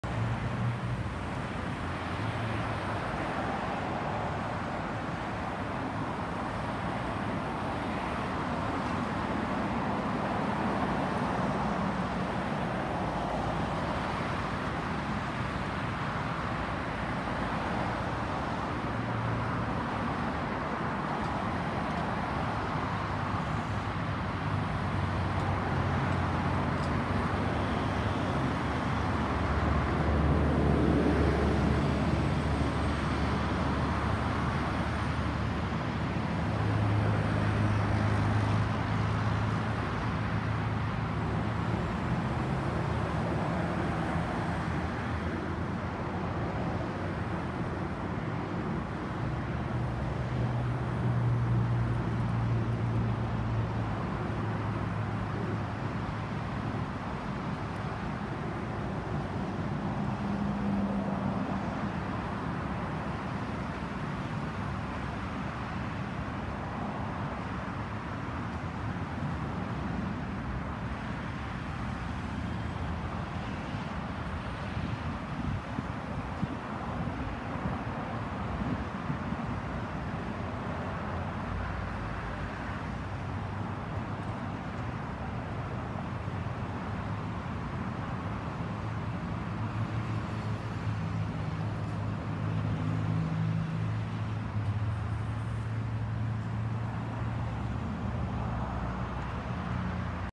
Thai Lion Air Boeing 737-8GP(WL) HS-LUJ flyover
Thai Lion Air Boeing 737-8GP(WL) (B738) HS-LUJ flyover in Bangkok (SL792 DMK-NST)